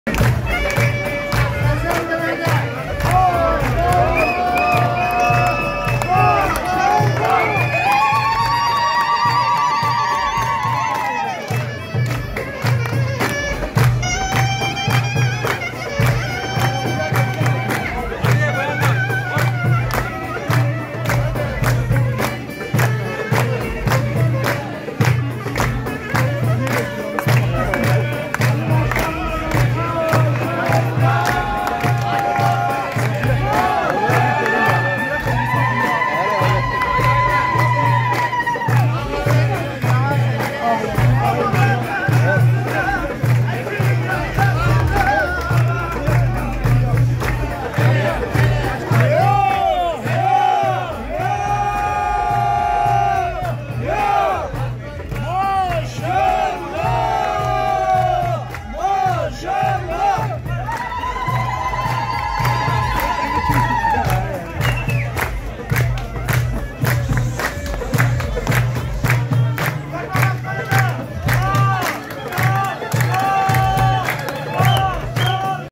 Cihan ve Aslan böyle oynadı
Gaziantep Büyükşehir Belediye başkanlığını farklı kazanan Fatma Şahin ve ekibi zafer böyle kutlaması yaptı. Büyükşehir Belediyesi Genel Sekreteri Sezer Cihan, Büyükşehir Belediyesi Genel Sekreter Yardımcısı Ali Aslan Antep Türküleri eşliğinde adeta kurtlarını döktü.